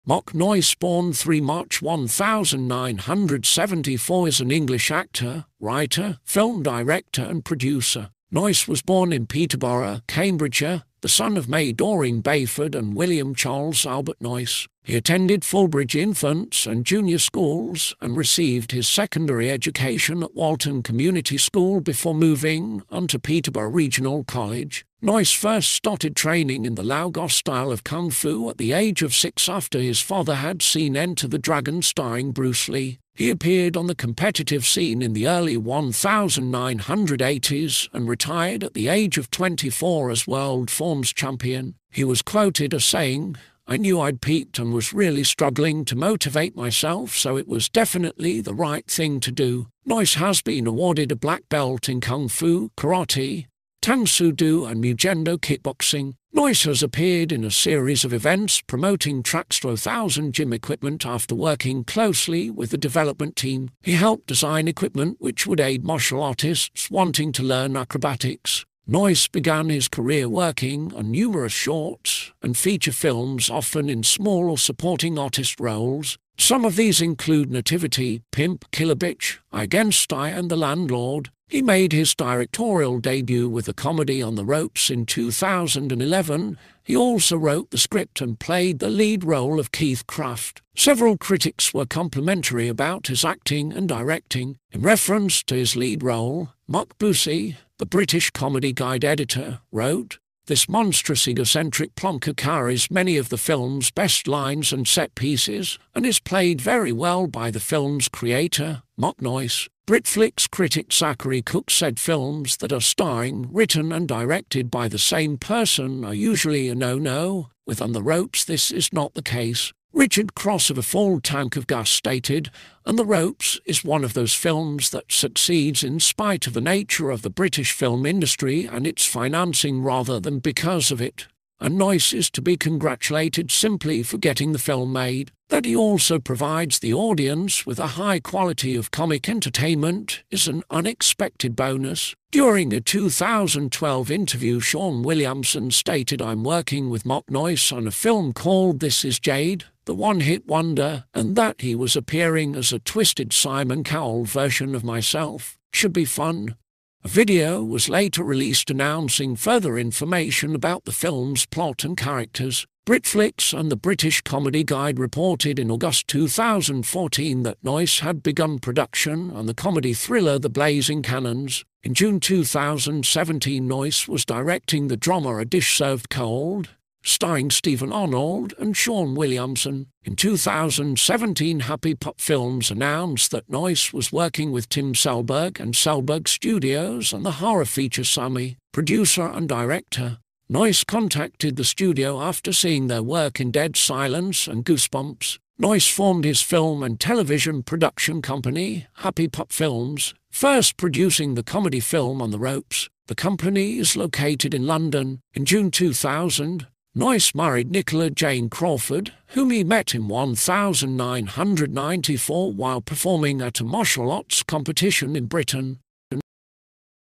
Mark Noyce Biography
This video is about Mark Noyce Biography in English.
Mark Noyce (born 3 March 1974) is an English actor, writer, film director and producer.
Noyce was born in Peterborough, Cambridgeshire, the son of May Doreen (Bayford) and William Charles Albert Noyce. He attended Fulbridge infants and junior schools and received his secondary education at Walton Community School before moving on to Peterborough Regional College.
Noyce first started training in the Lau Gar style of Kung-Fu at the age of 6 after his father had seen Enter the Dragon starring Bruce Lee. He appeared on the competitive scene in the early 1980s and retired at the age of 24 as world forms champion. He was quoted as saying, "I knew I’d peaked and was really struggling to motivate myself so it was definitely the right thing to do."
Noyce has been awarded a black belt in kung fu, karate, tang soo do and Mugendo kickboxing.
Noyce has appeared in a series of events promoting Tracks2000 gym equipment after working closely with the development team. He helped design equipment which would aid martial artists wanting to learn acrobatics.
Noyce began his career working on numerous shorts and feature films often in small or supporting artist roles. Some of these include Nativity, Pimp, Killer Bitch, I against I and The Landlord.
He made his directorial debut with the comedy On the Ropes in 2011, he also wrote the script and played the lead role of Keith Kraft.
Several critics were complimentary about his acting and directing. In reference to his lead role, Mark Boosey, The British Comedy Guide editor, wrote, "This monstrous egocentric 'plonker' carries many of the film's best lines and set pieces, and is played very well by the film's creator, Mark Noyce". Britflicks critic Zachary Cooke said "films that are starring, written and directed by the same person are usually a no no, with on the Ropes this is not the case". Richard Cross of A Full Tank of Gas stated, "On the Ropes is one of those films that succeeds in spite of the nature of the British film industry and its financing rather than because of it, and Noyce is to be congratulated simply for getting the film made. That he also provides the audience with a high quality of comic entertainment is an unexpected bonus".
During a 2012 interview Shaun Williamson stated "I'm working with Mark Noyce on a film called This Is Jayde: The One Hit Wonder" and that he was "appearing as a twisted Simon Cowell version of myself. Should be fun!" A video was later released announcing further information about the film's plot and characters.
Britflicks and The British Comedy Guide reported in August 2014 that Noyce had begun production on the comedy thriller The Blazing Cannons.
In June 2017 Noyce was directing the drama A Dish Served Cold, starring Steven Arnold and Shaun Williamson.
In 2017 Happy Pup Films announced that Noyce was working with Tim Selberg and Selberg Studio's on the horror feature Sammy. Producer and director, Noyce contacted the studio after seeing their work in Dead Silence and Goosebumps.
Noyce formed his film and television production company, Happy Pup Films, first producing the comedy film On the Ropes. The company is located in London.
In June 2000, Noyce married Nicola Jayne Crawford, whom he met in 1994 while performing at a martial arts competition in Britain.